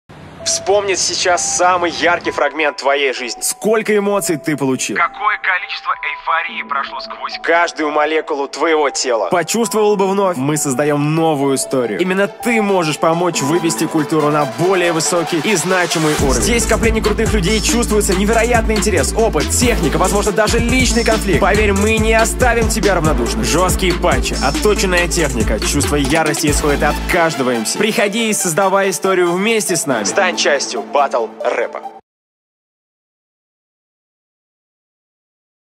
BPM RAP BATTLE | Нижний Новгород | Тизер
Мы первые, кто вносит вклад в данную  культуру в этом городе.
Будь внутри нижегородского  баттл рэпа и твори историю вместе с нами.
BPM-вместо одного 140 шагов вперёд!

24 ноября 17:00 FIDEL CLUB. Мы ждём именно тебя